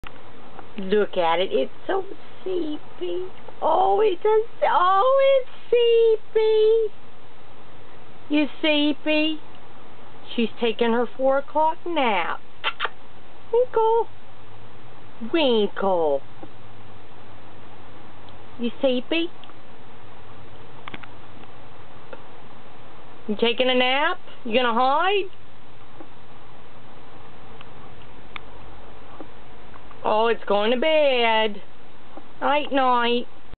Winkelhimer Smith in Seepy Squirrel
My Squirrel Winkelhimer Smith about to take a nap.You can see she pretty much chewed the hell out of her wicker shelf she sleeps on.Her tongue looks like a ant eater tongue when she yawns.I got her when she was a toddler one of my outside cats tried to eat her so I nursed her back to health.And the arm he bit her on turned out cripple so we just kept her as a house pet.She just a TINY bit spoiled. Ugly Shyla